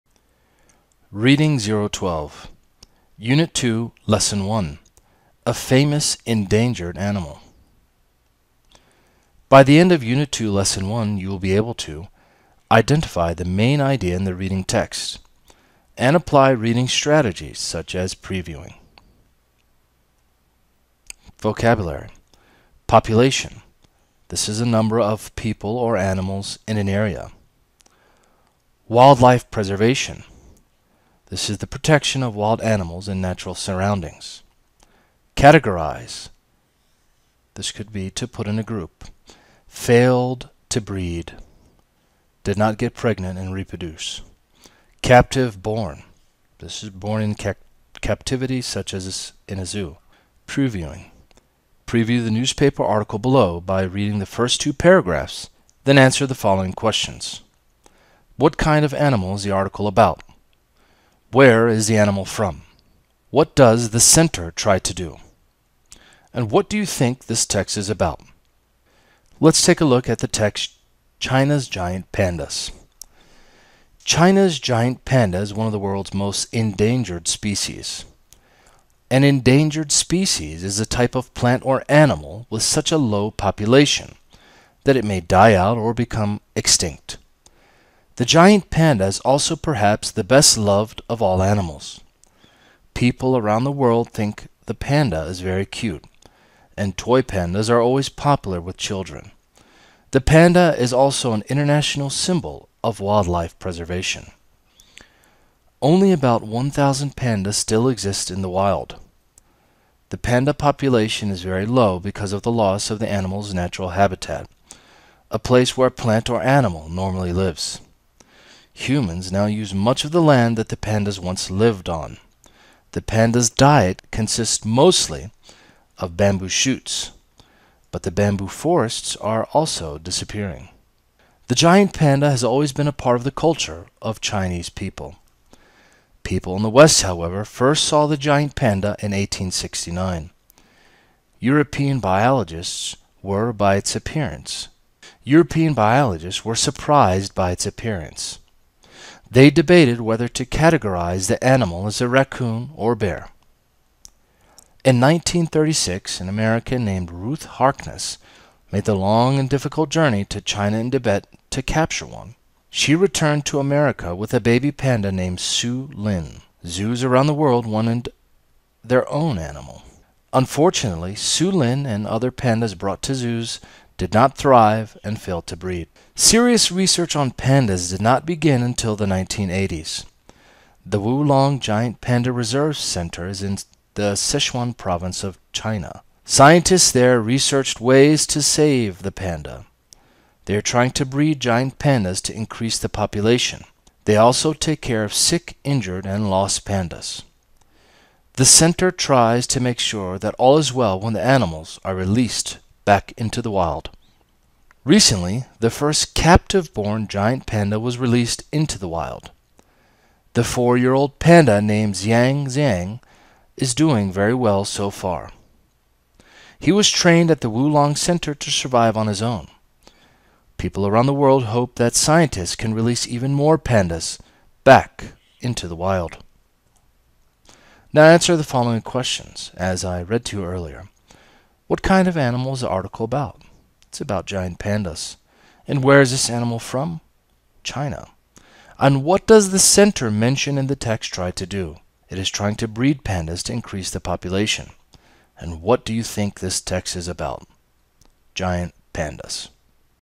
Intensive English | Reading U2-L1 | A Famous Endangered Animal
المقررات المفتوحة - مقرر Intensive English
Reading  U2-L1A Famous Endangered Animal
Identify the Main Idea in the reading text.
Apply reading strategy – Previewing.